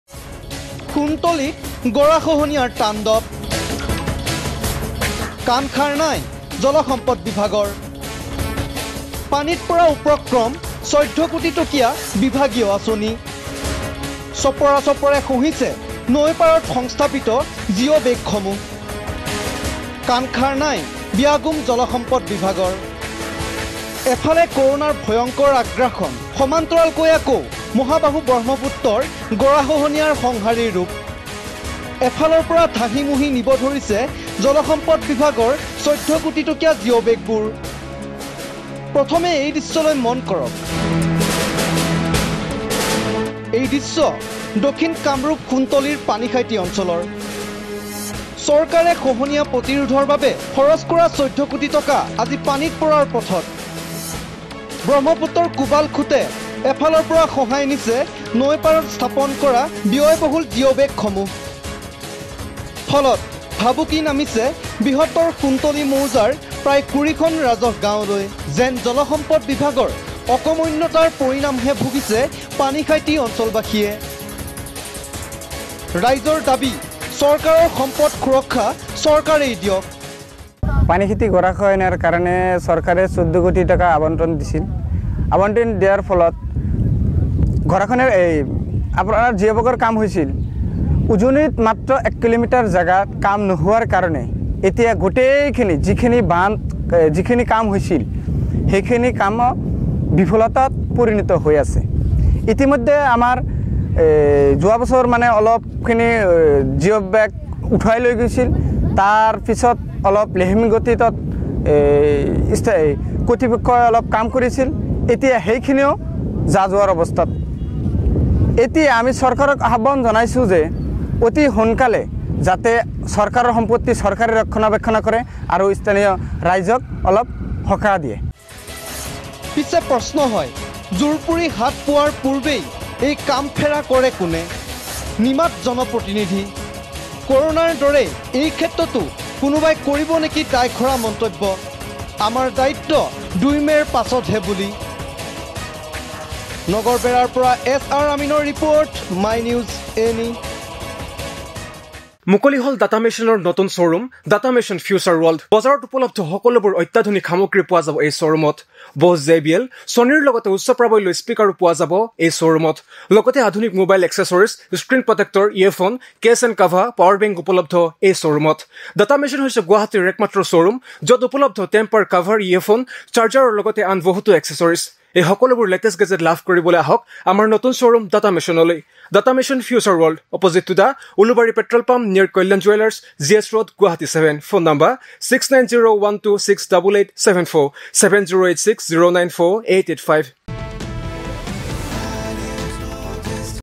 সোণতলীত গৰাখহনীয়াৰ তাণ্ডৱ কাণ সাৰ নাই জলসম্পদ বিভাগৰ
My News NE:
◆সোণতলীত গৰাখহনীয়াৰ তাণ্ডৱ
◆কাণ সাৰ নাই জলসম্পদ বিভাগৰ
◆পানীত পৰাৰ উপক্ৰম ১৪ কোটি টকীয়া বিভাগীয় আঁচনি
About Channel: